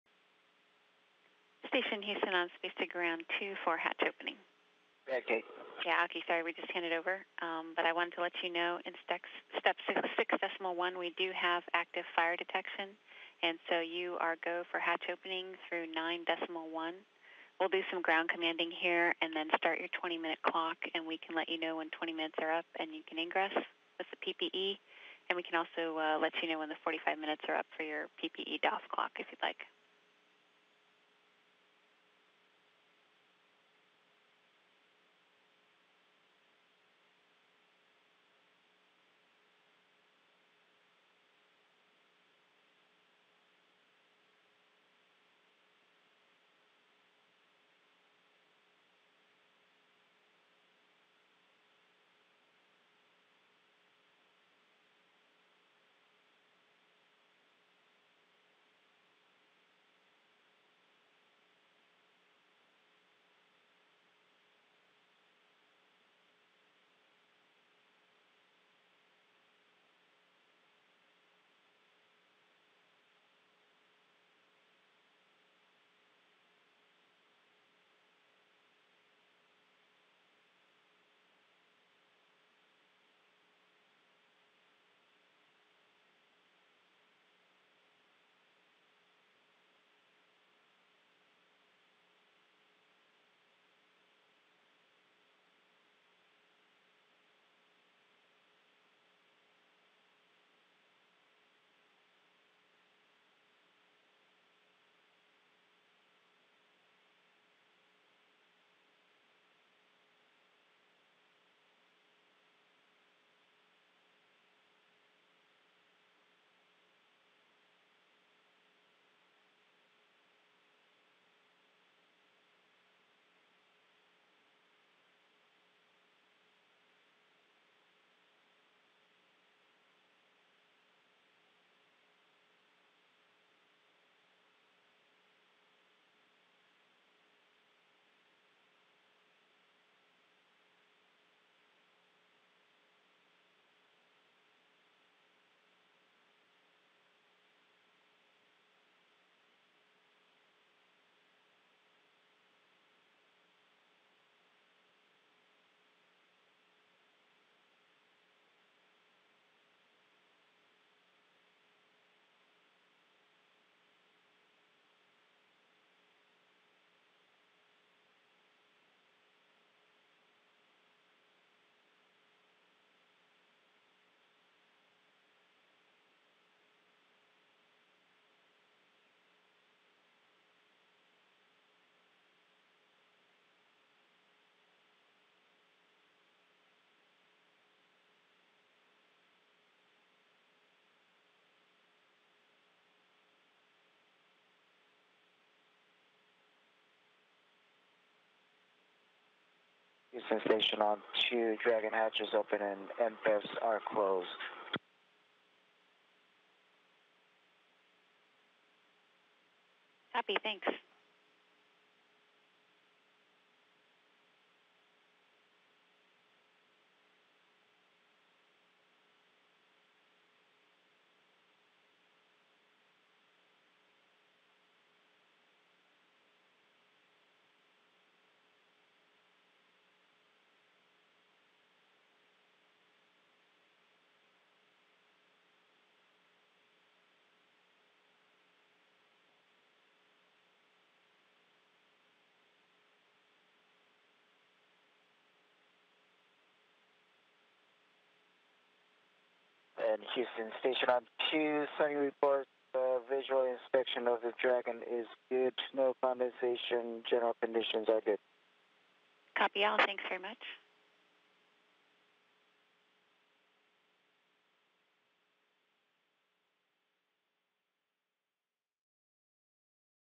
Station Crew Opens Dragon Hatch
Expedition 33 Commander Suni Williams and Flight Engineer Aki Hoshide opened the hatch to the SpaceX Dragon cargo ship at 1:40 p.m. EDT Wednesday, Oct. 10, marking a milestone for the first commercial resupply mission to reach the International Space Station.